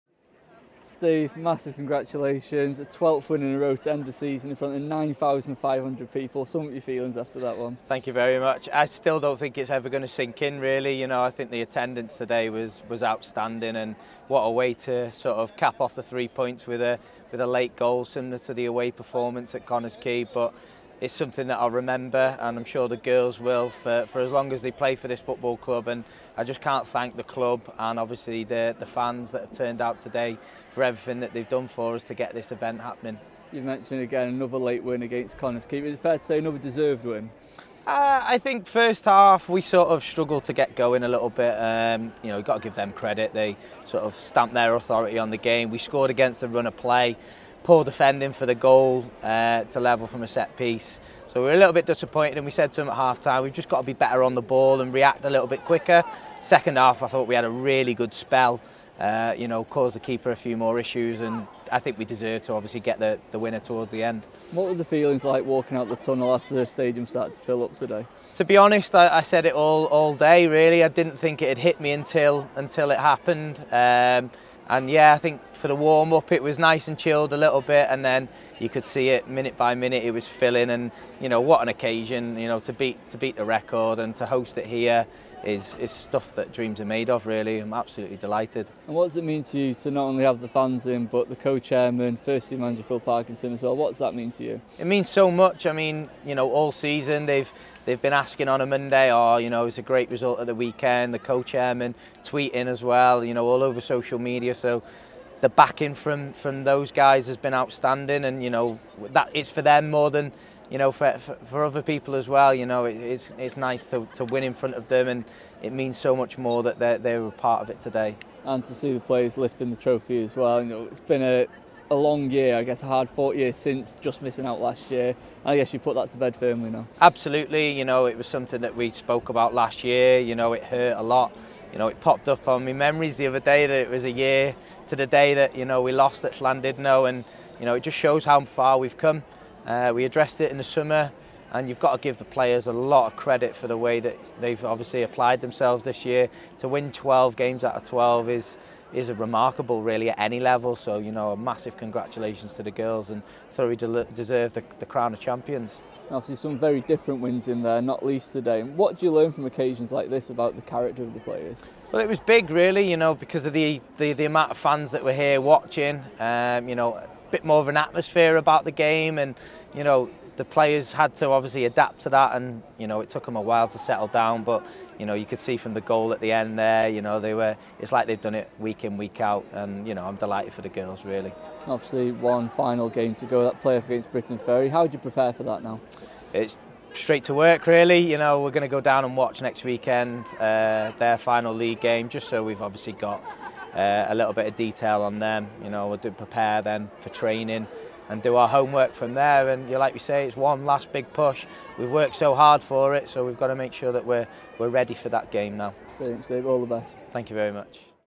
INTERVIEW | Steve Dale after Wrexham AFC Women vs Connah's Quay Nomads
Manager Steve Dale looks back on a record-breaking day at the Racecourse Ground